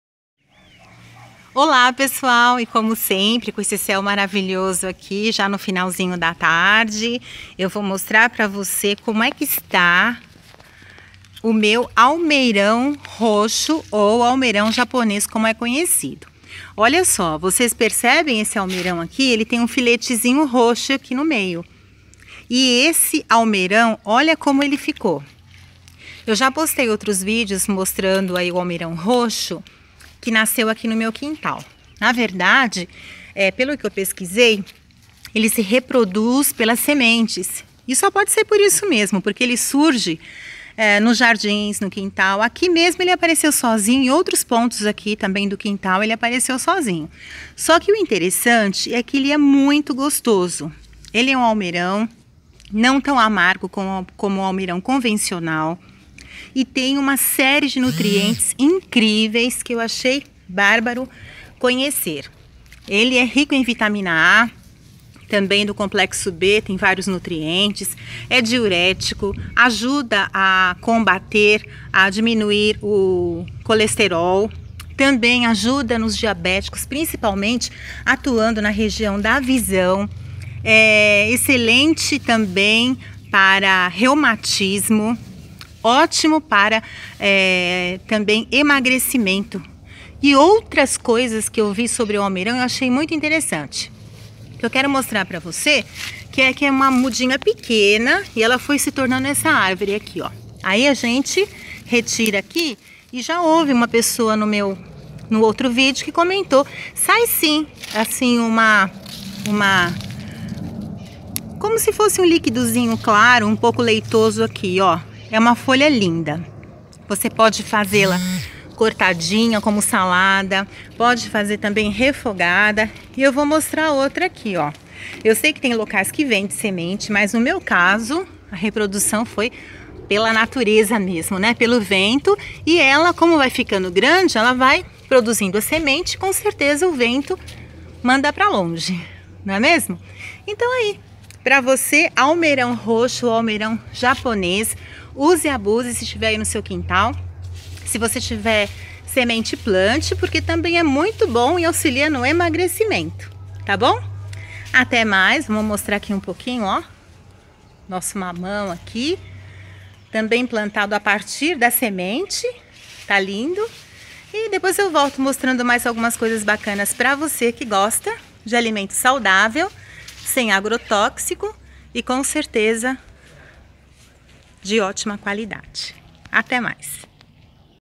ALMEIRAO Roxo ou Japonês
Veja o Almeirão Japonês ou Almeirão Roxo. Muitos nutrientes e ótimo para emagrecimento, diabetes, ajuda a diminuir o colesterol e você pode ter aí no seu quintal ou jardim sem saber.